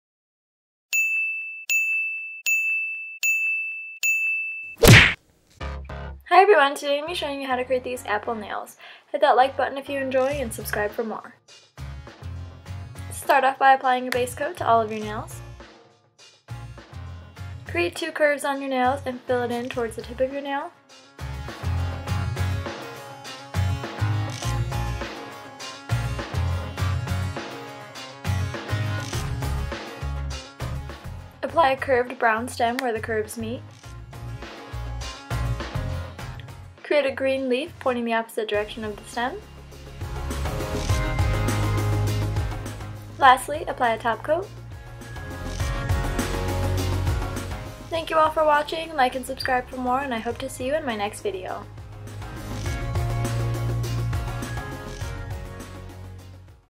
Apple Nails
Hi everyone! This weeks tutorial is for Apple Nails! These can be used as some back to school nails or just some fruit nails. These are super easy, quick and simple! I hope you all enjoy!

Base Coat: Orly's "Bonder Rubberized Base Coat"
Top Coat: Orly's "Polishield All-In-One Ultimate Top Coat"
Polishes Used: No Brand No Name bright red polish and Sally Girl's "812165"
Acrylic Paints Used: Delta Ceramcoat's "Spice Brown" and Craft Smart's "Crisp Green"
Tools Used: a welding tool or toothpick